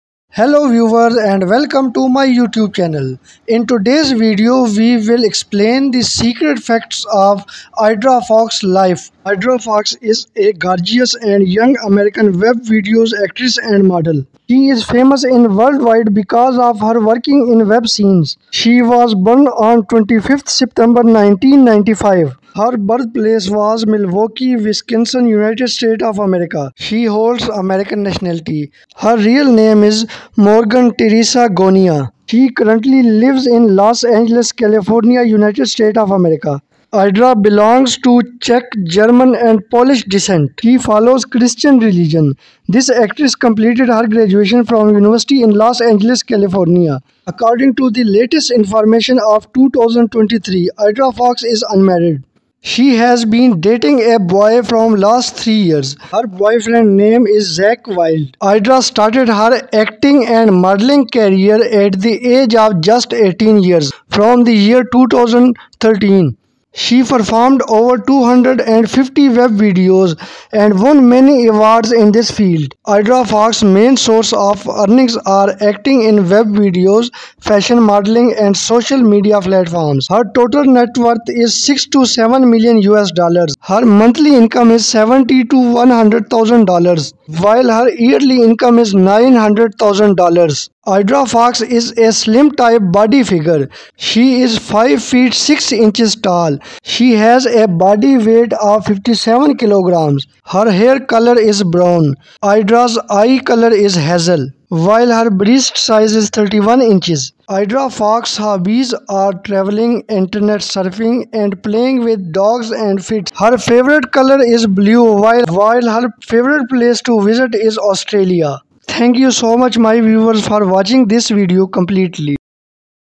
US actress Aidra Fox biography and lifestyle | net worth | boyfriend | age
This is an informational video. In this informative video viewers will know about the American web series actress Aidra Fox biography in English, secrets and facts.
Aidra Fox biodata.